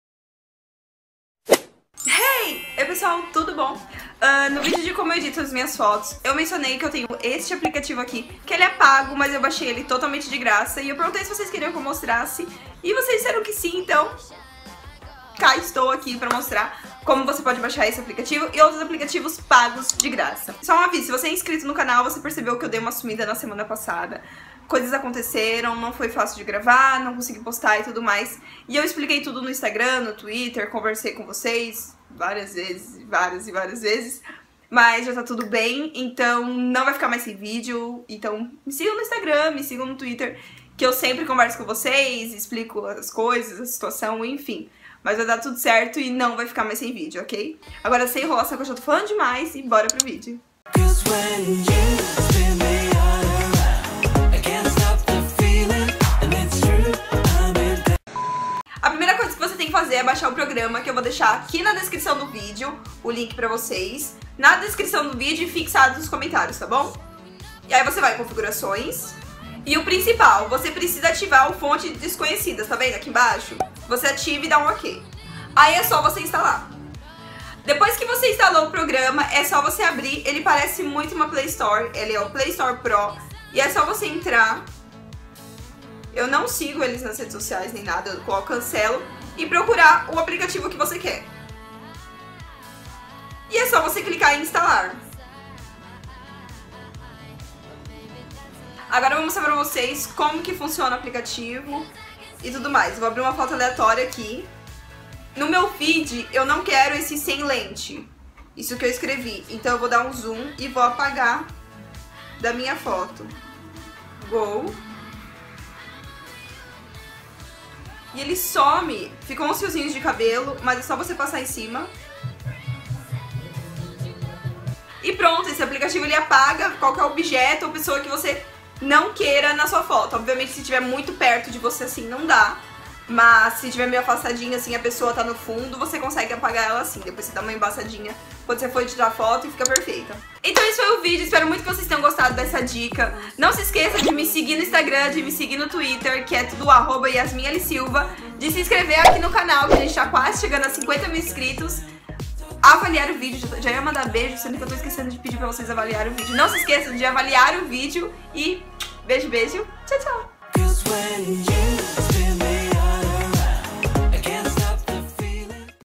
COMO BAIXAR APPS PAGOS DE GRAÇA | YASMIN L. SILVA
Heeey, hoje eu vim contar como você pode baixar aplicativos pagos de graça, essa dica me ajudou muito e espero que vocês gostem, beiju beiju, amo vocês.